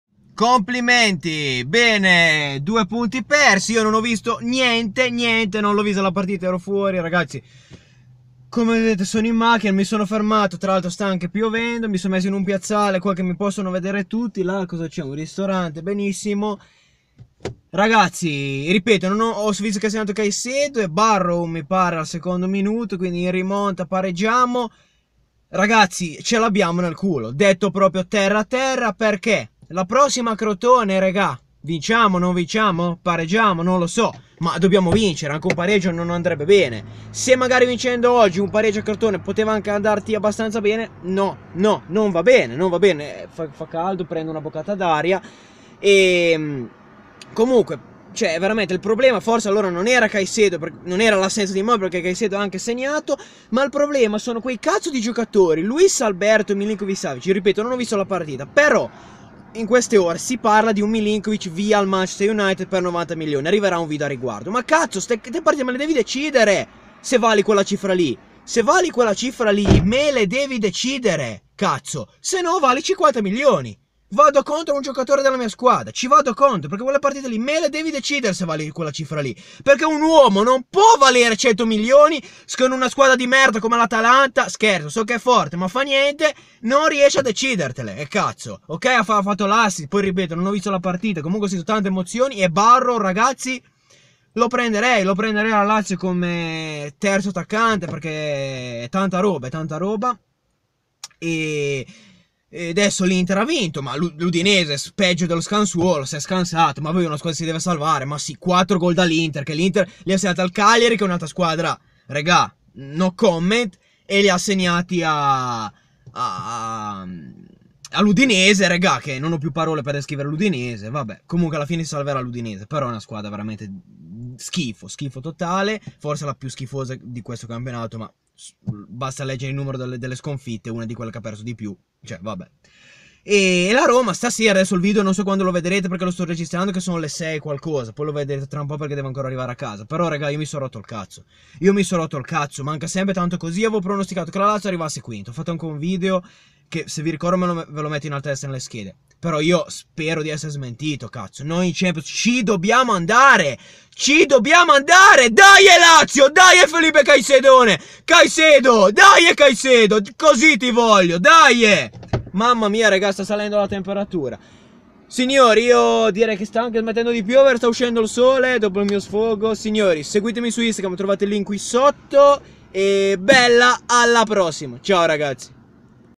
[SFOGO]LAZIO-ATALANTA1-1: MI SONO ROTTO LE PALLE! - La Rabbia di un Tifoso Laziale
Quando è ora di vincere non vinciamo mai!!!